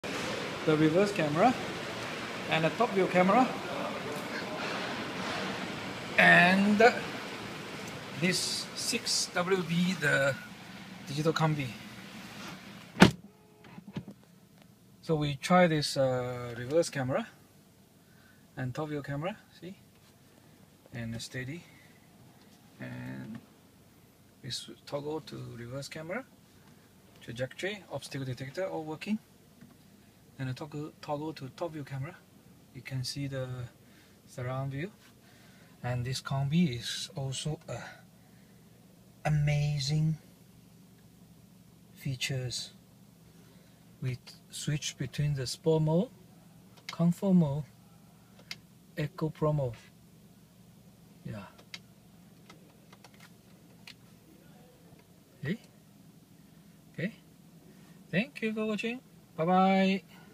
Digital Cluster DKombi 6WB and Top View and Reverse View Camera
Retrofit of 6WB and Top View Cameras